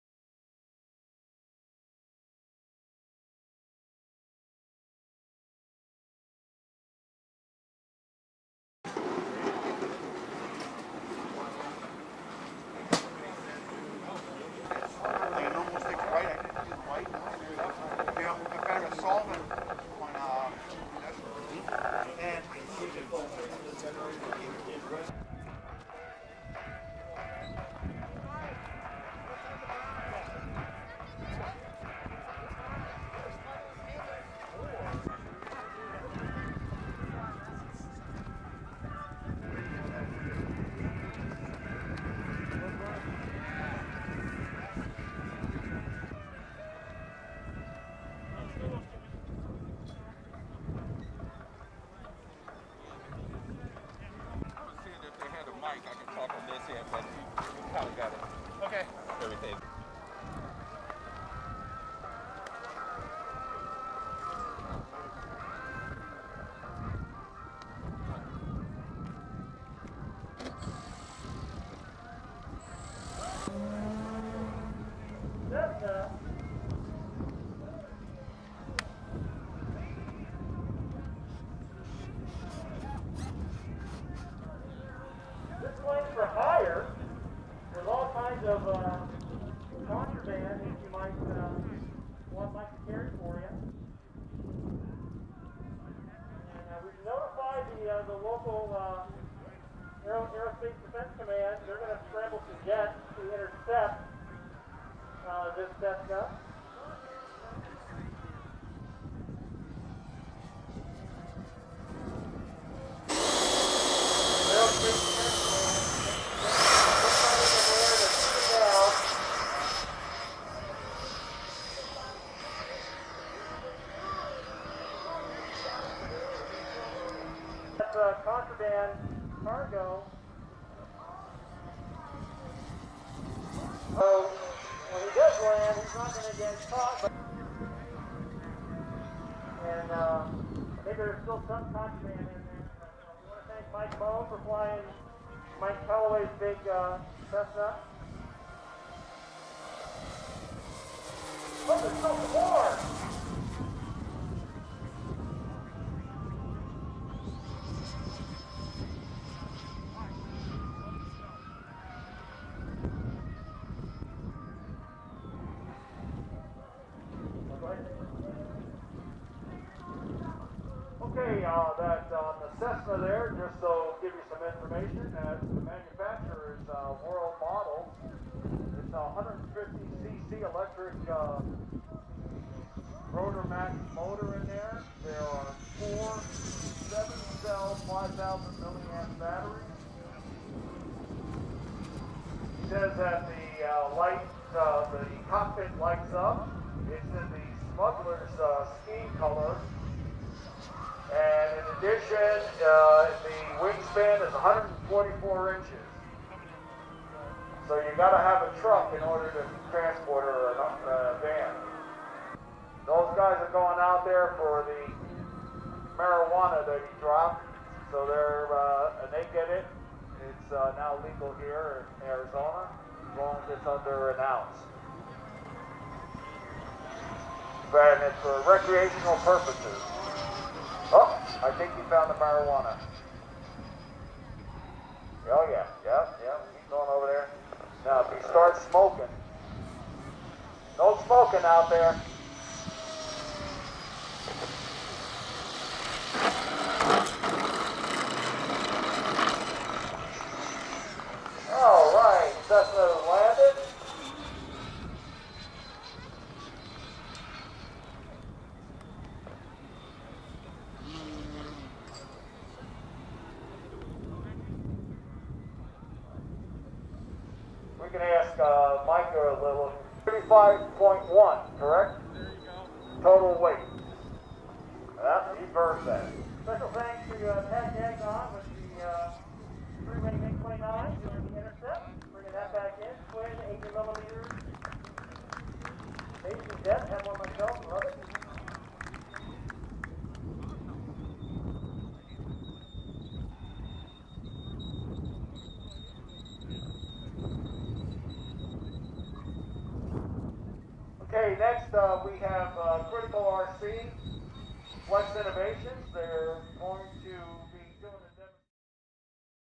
Giant Electric Cessna 172 144" wingspan
Some of my video footage of Michael Callaway's 144in wingspan World Models Cessna 172 at the Arizona Electric Festival 2021. The Cessna is running a Turnigy RotoMax 150cc size brushless outrunner motor. The ESC is a Turnigy 250 Amp 14S. Batteries are 4 zippy 5,000mAh 7S lipos. Weight is 35lbs. CORRECTION THE OWNER IS MICHAEL CALLOWAY.  PILOT MIKE MOHN.